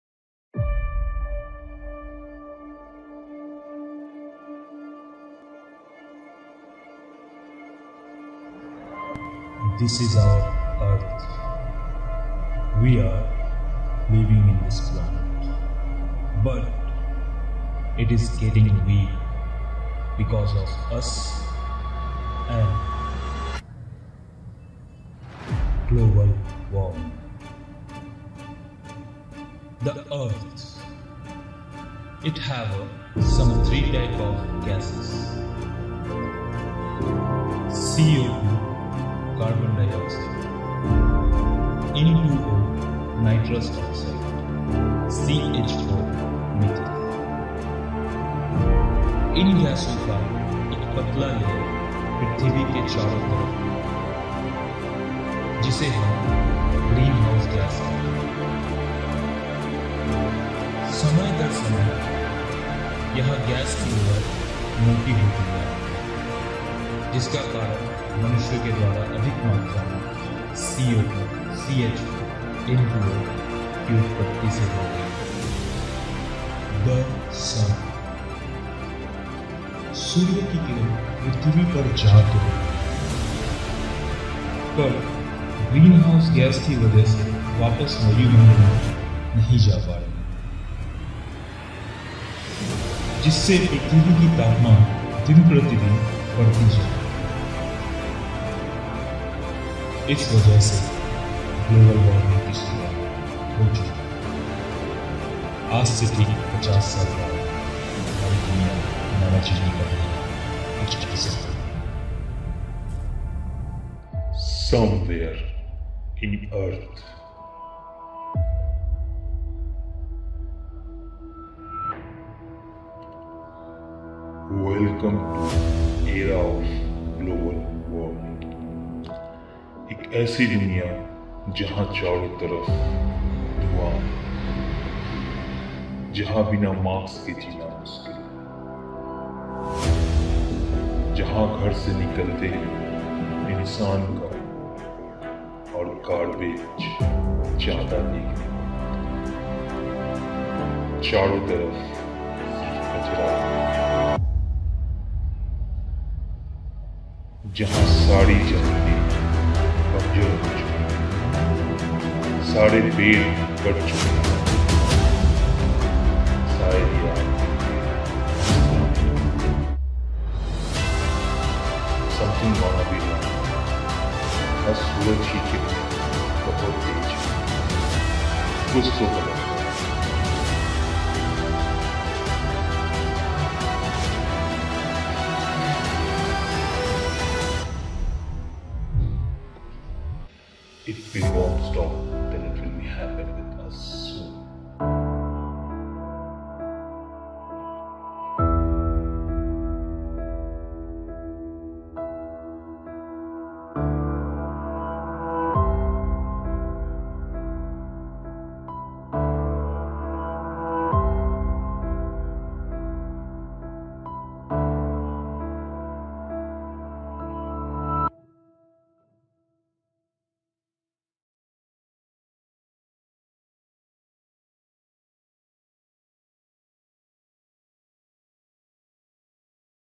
GLOBAL WARMING SHORT FILM || P.S ENTERTAINMENT PRESENTS //
THIS VIDEO DESCRIBE HOW WE ARE GOING TO END. HOW MUCH INCREMENT OF GLOBAL WARMING. IF WE DON'T STOP RIGHT NOW THEN IN FUTURE WE WILL PAY FOR IT.
THIS VIDEO BASED ON AWARENESS OF GLOBAL WARMING..

DIRECTED BY : PRABHAT SONKAR
FEATURING :. MANAV SONKAR
VFX , VOICE OVER, EDITED BY PRABHAT SONKAR
P S ENTERTAINMENT PRESENTS